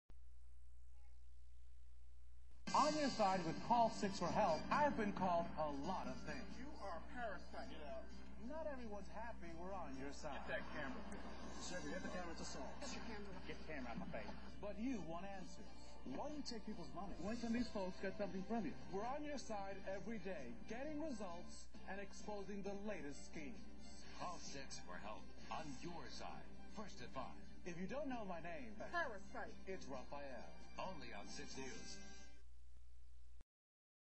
Call 6's Rafael Sanchez Has Been Called A Lot Of Things....
People have choice words for a local Indianapolis TV journalist.  At least, he doesn't suffer from low self-esteem.